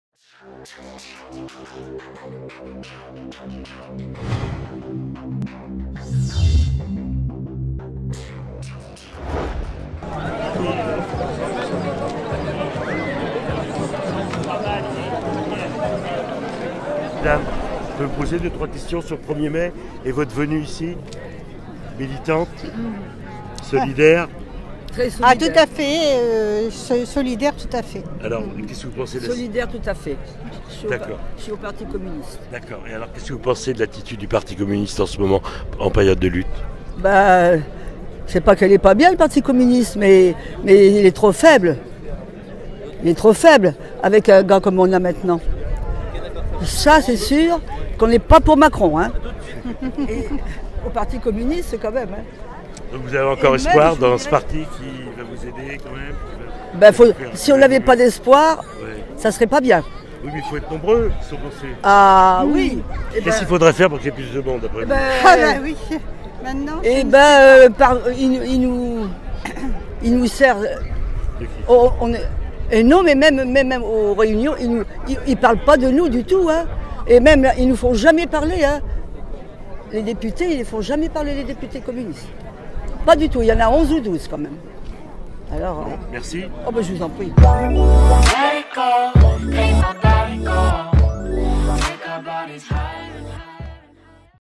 (2) Elles ont tenu à célébrer le 1er mai à Rochefort et manifester leur colère
Elles ont tenu à célébrer le 1er mai à Rochefort et manifester leur colère contre la casse du service public . (Video 2)